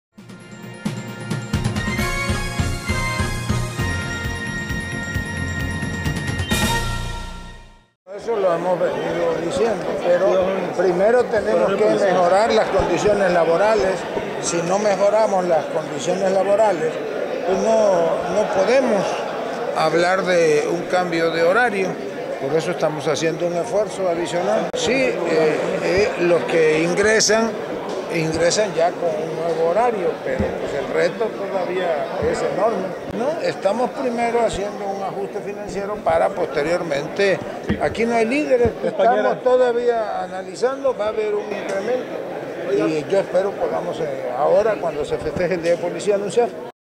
AALH MEJORAS A POLICÍAS
NoticiasTVT El gobernador Adán Augusto López Hernández buscará mejorar las condiciones laborales de los policías y posteriormente abordará el tema del cambio de horario